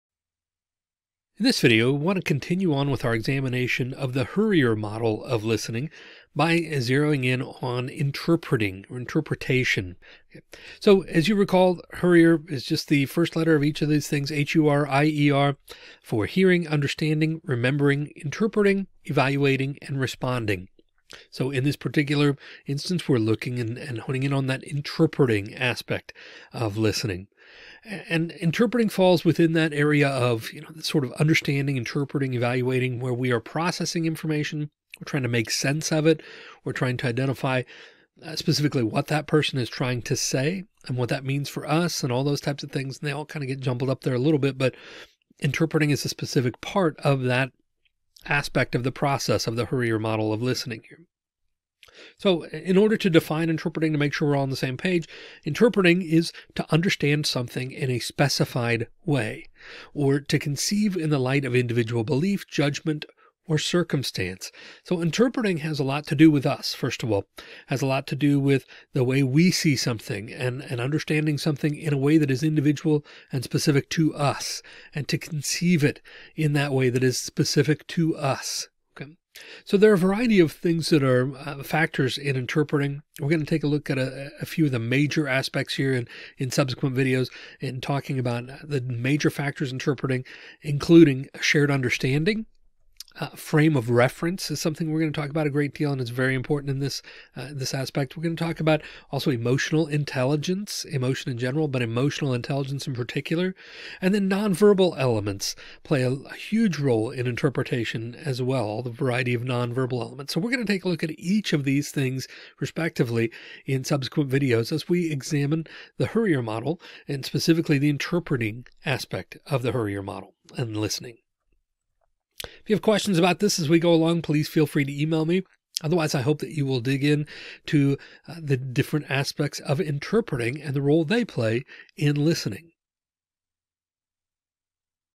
The HURIER Model: Interpreting
Introduction to the fourth component of the HURIER model of listening - Interpreting.  Defines the term and lays out the constituent elements to be explored including frame of reference, emotional intelligence, and nonverbal communication.